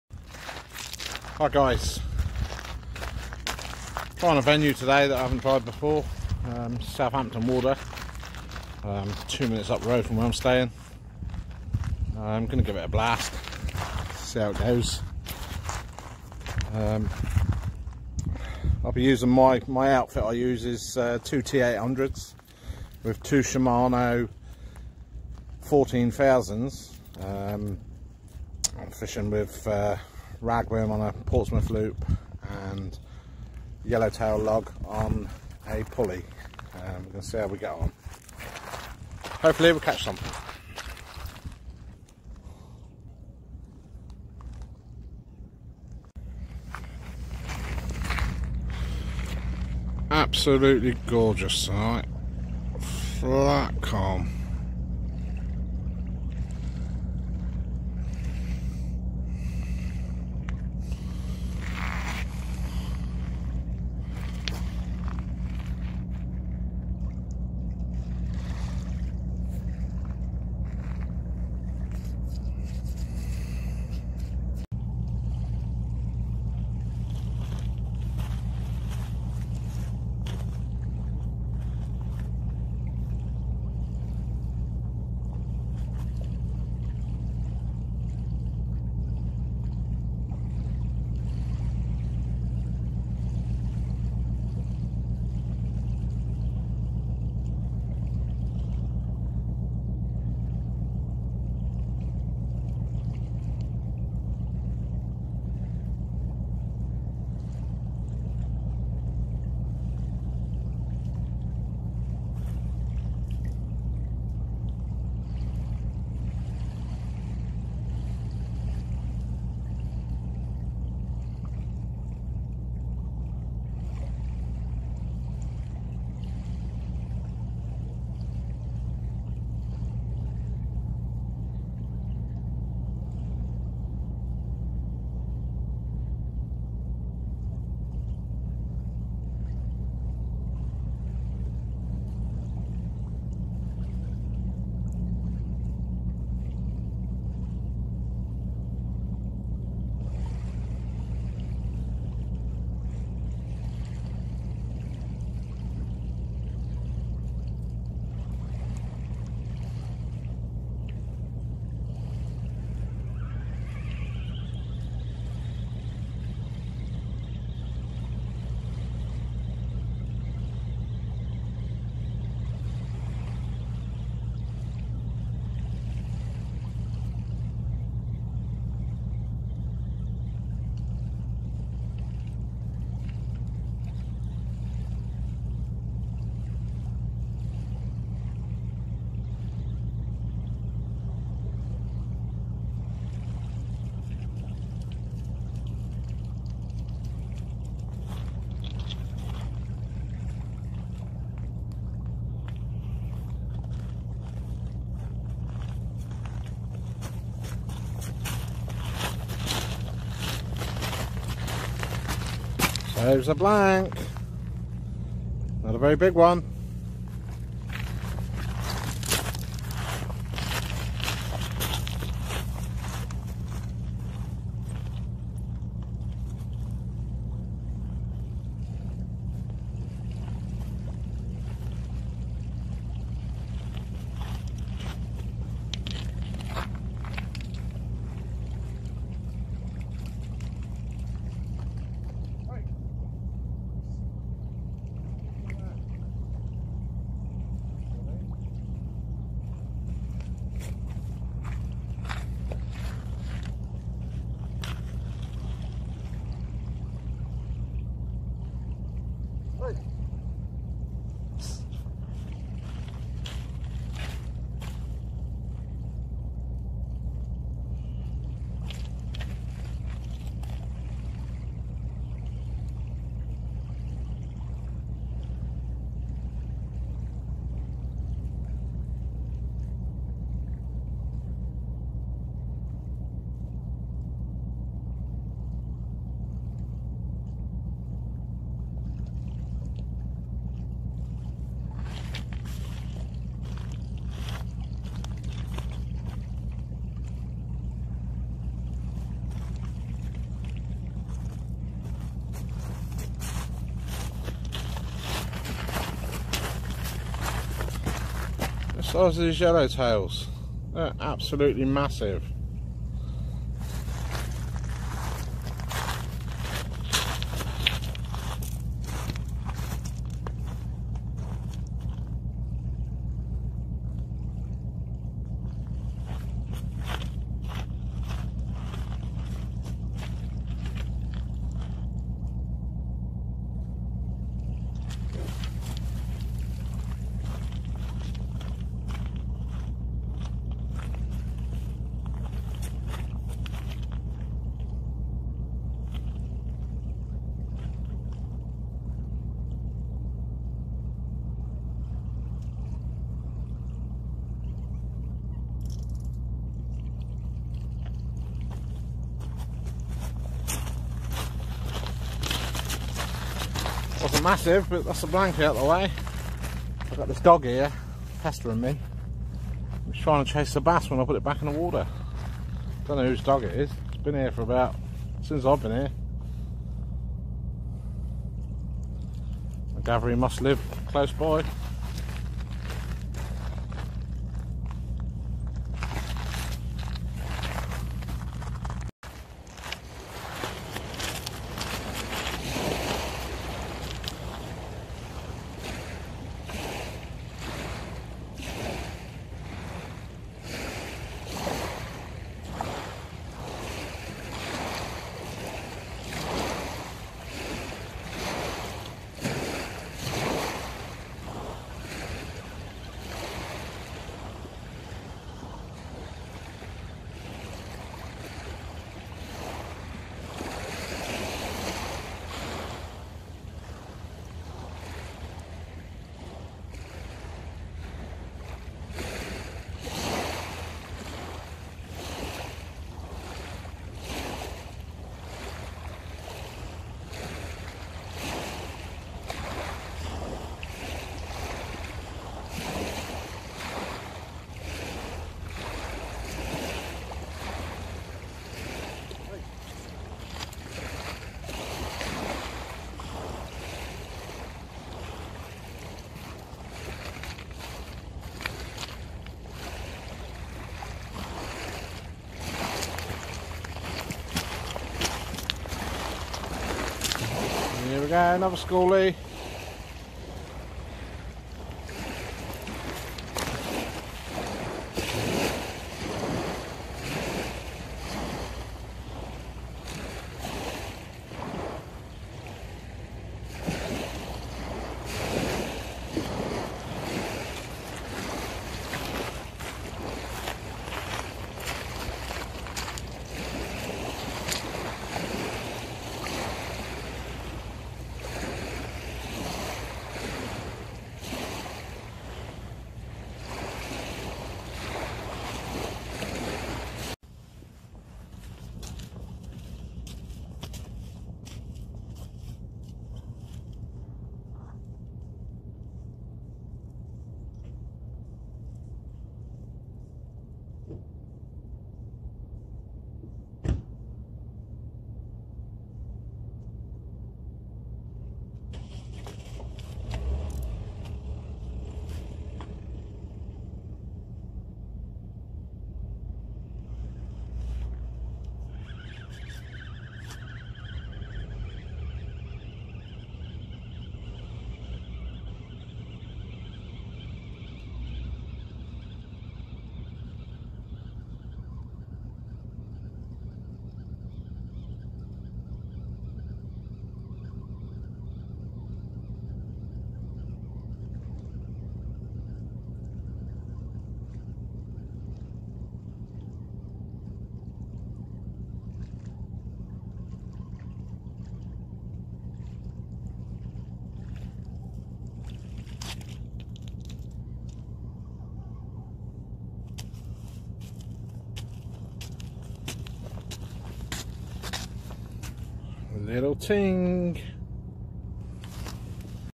Beach Lane January 2021
These are a few clips from my trip to Beach Lane.  Thank you and stay safe 🎣